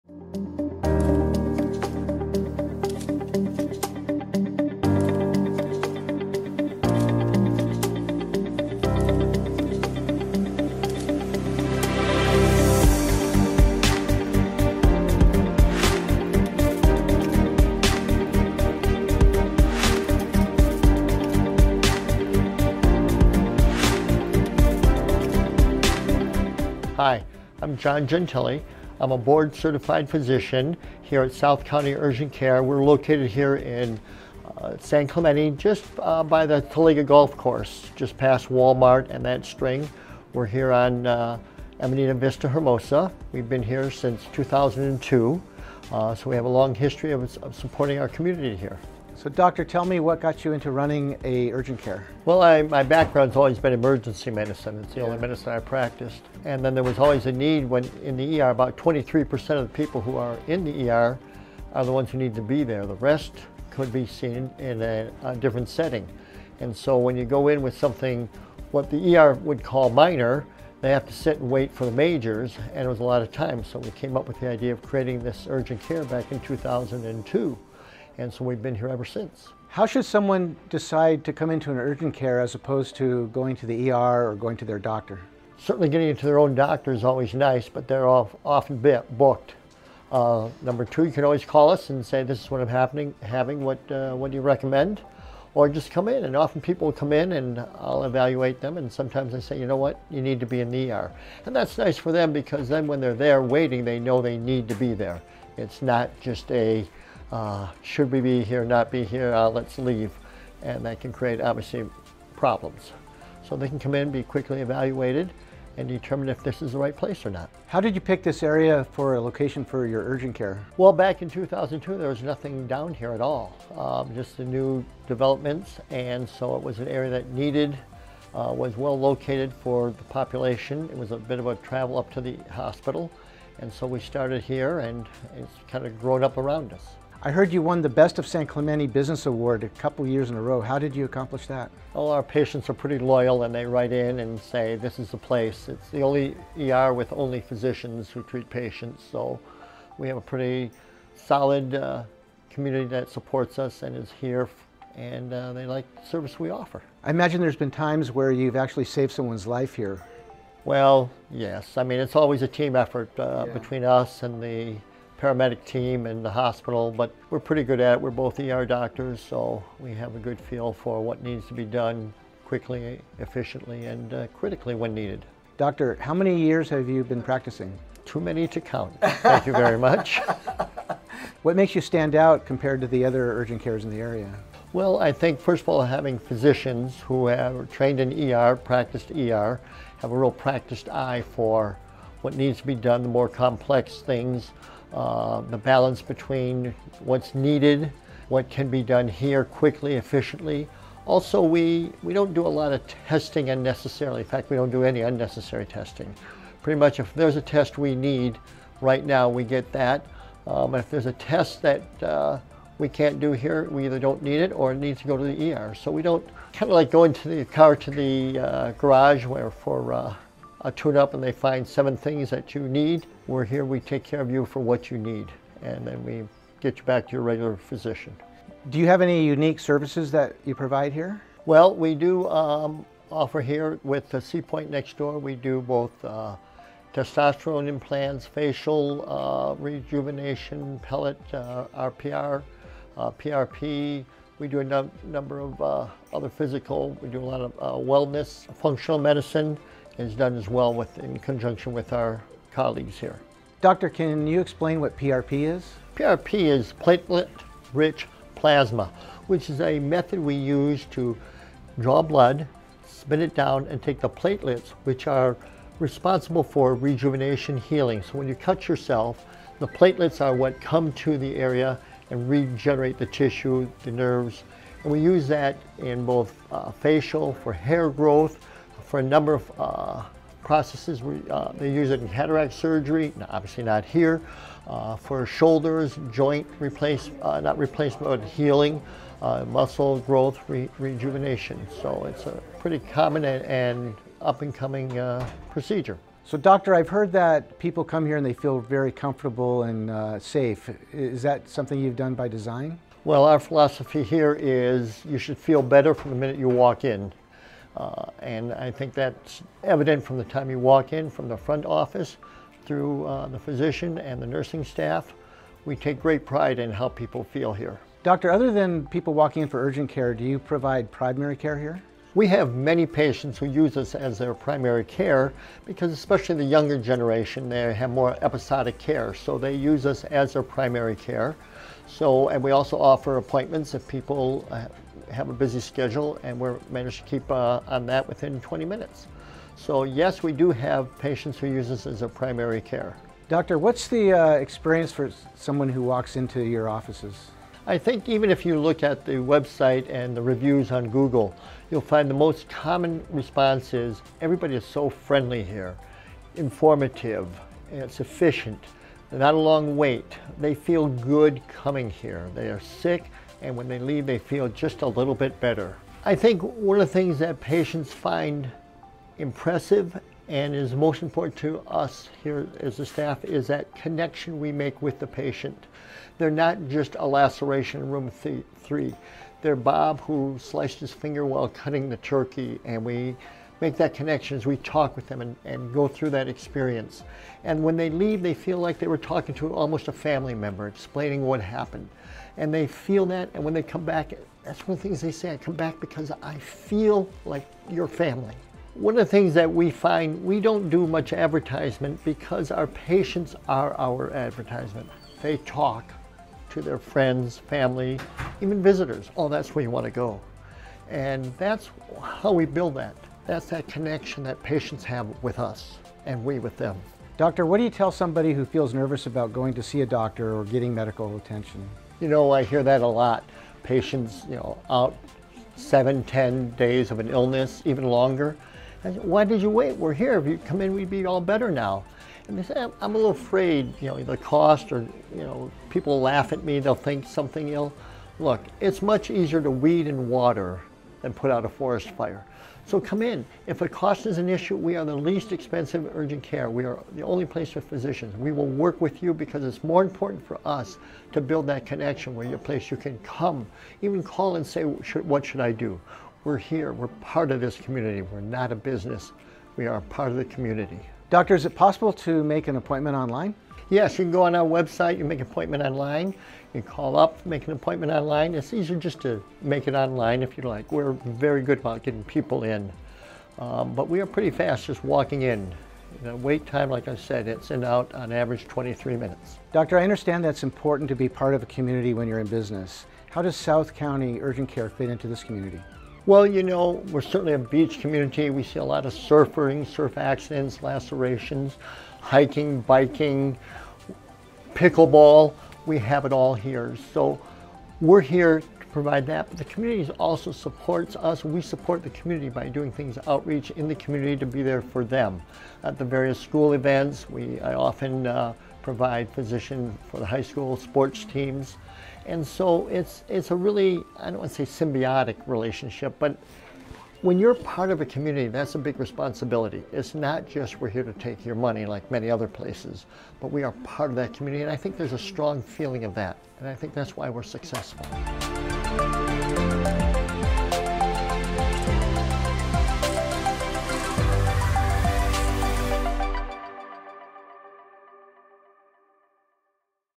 San Clemente's Affordable Urgent Care: A Community Hero Story
🎬 Join Beyond Magic Films in uncovering the heartwarming journey of Dr. John, a local hero in San Clemente. Teaming up with realtor Tony English, we showcase Dr. John's affordable urgent care and his impactful role in the community. Witness the power of compassion and community unity in this inspiring tale of hope and healing.